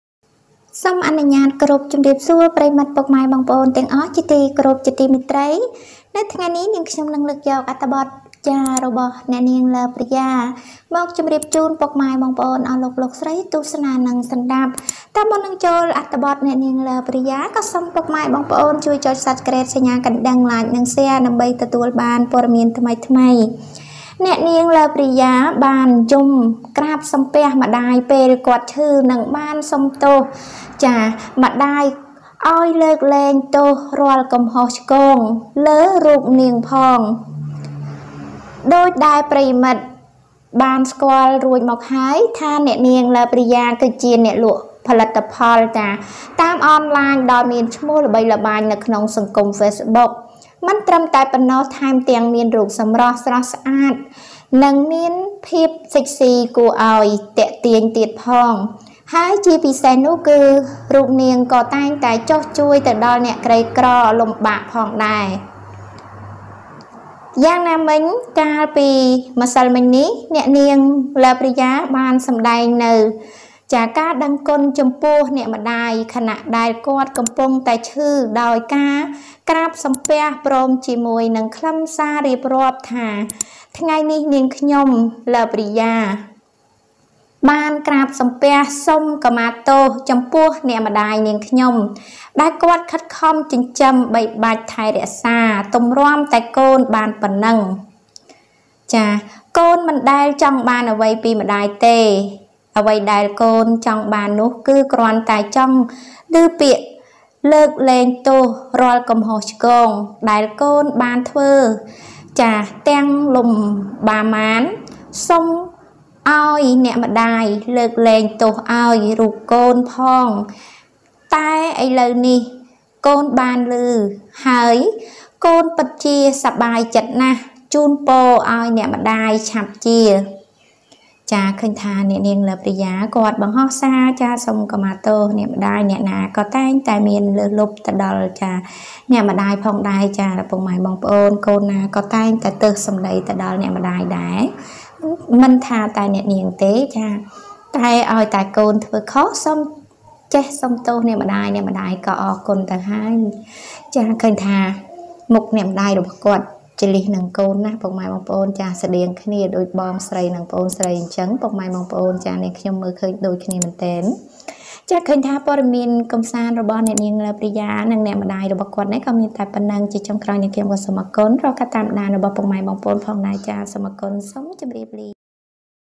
អ្នកនាង​ Love Riya យំក្រាប​សំពះ​សុំខមាទោសម្តាយ​ពេលគាត់ឈឺ​​ និងបាននិយាយ​សុំ​ម្ដាយ លើកលែងទោសឲ្យរូបនាងផ
Love Riya cries out to her mother when she is sick and begs her pardon

អ្នកនាង​ Love Riya យំក្រាប​សំពះ​សុំខមាទោសម្តាយ​ពេលគាត់ឈឺ​​ និងបាននិយាយ​សុំ​ម្ដាយ លើកលែងទោសឲ្យរូបនាងផង ​

channel/ChanthyNews